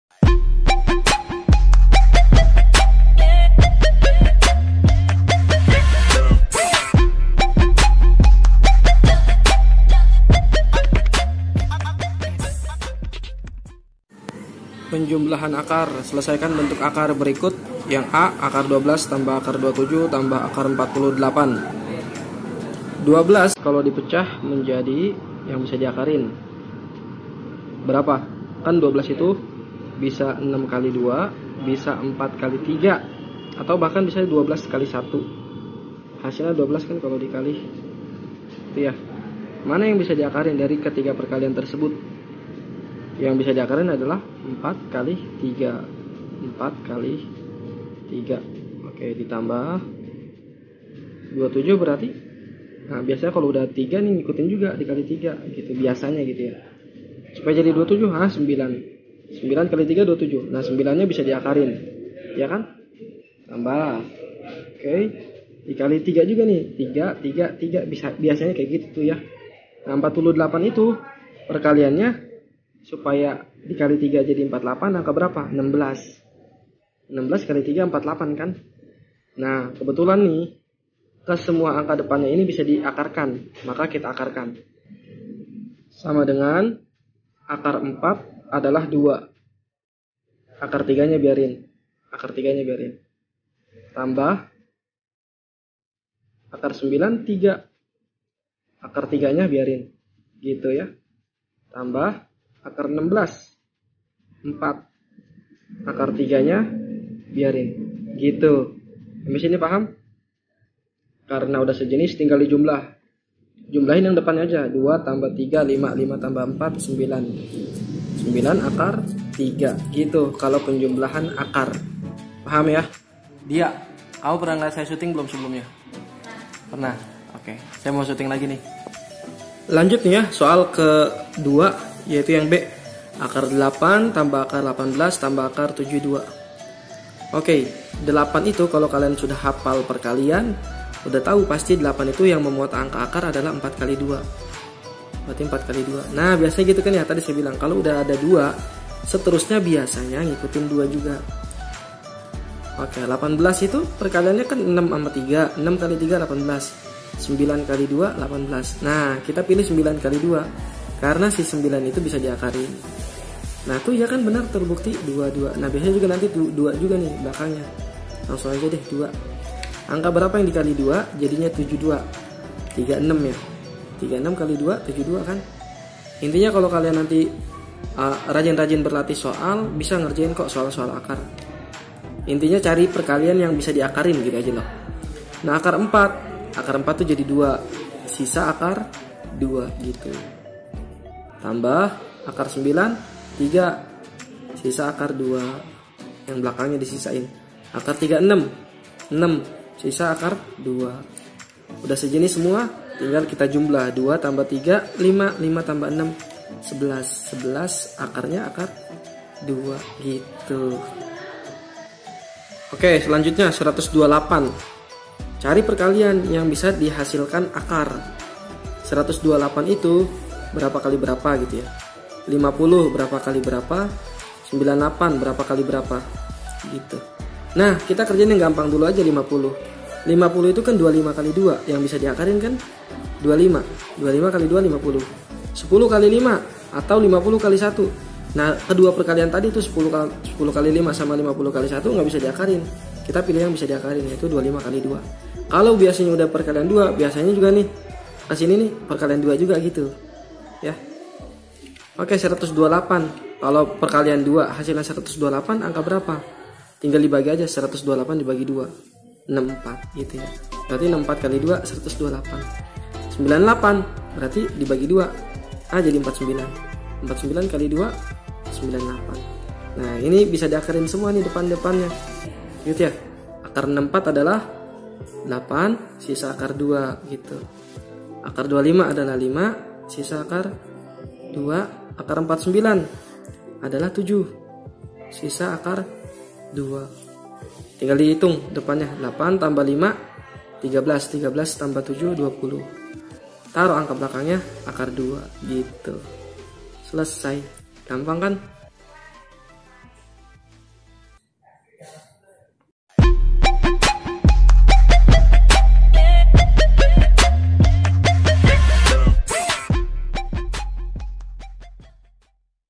PENJUMLAHAN AKAR
Penjumlahan Akar merupakan materi matematika kelas 9 SMP namun dikelas 10 SMA pun materi ini ada